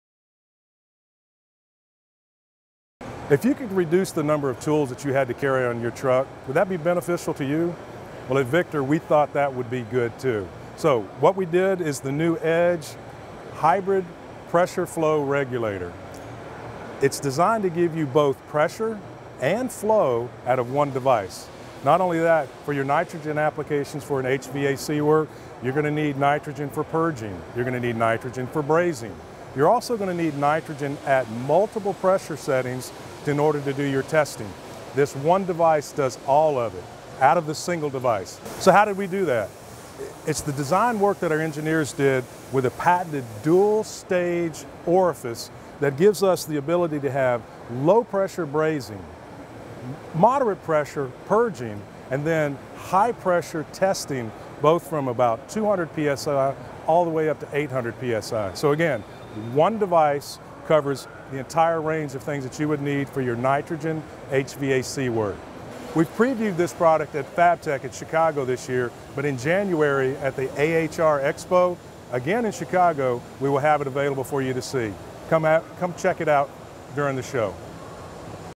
EDGE 2.0 Pressure/Flow Hybrid Regulator for HVAC Applications
Victor provided a preview of a hybrid regulator that combines functions for brazing, nitrogen purge and pressure testing in a single regulator.

The regulator will also be showcased at the AHR Expo, Jan. 22 - 24 at McCormick Place.